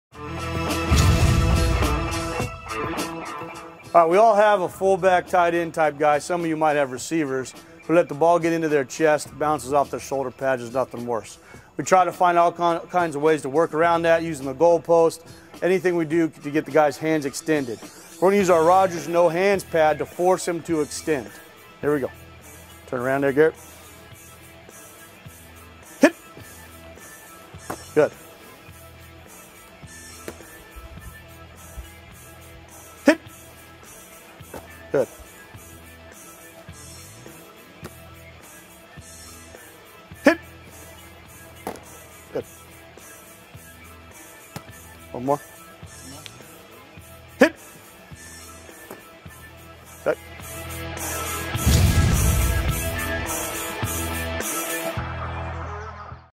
Extend to Receive
Pass catching drill using the No-Hands Pad.